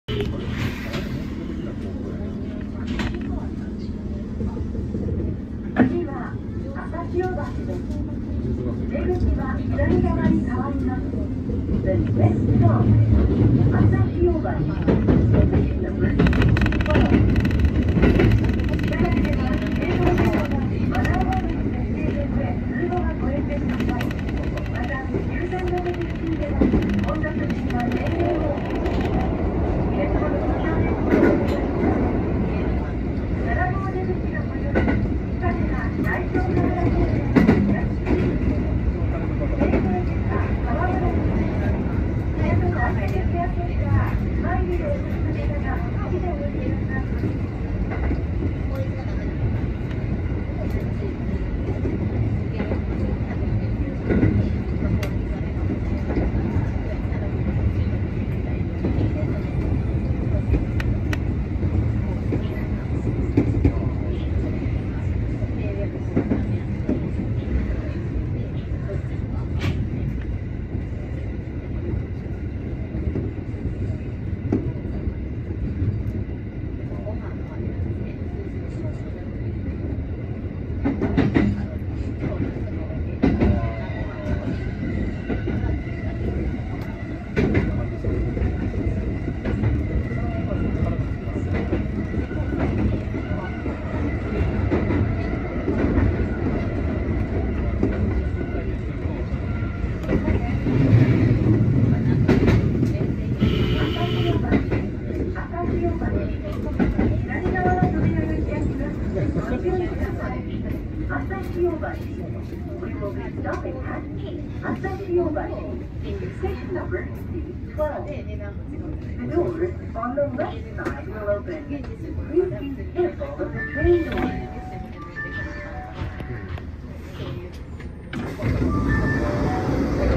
【前面展望】OsakaMetro中央線 大阪港→朝潮橋（大阪メトロ）
OsakaMetro（大阪メトロ）中央線
大阪港→朝潮橋の前面展望です。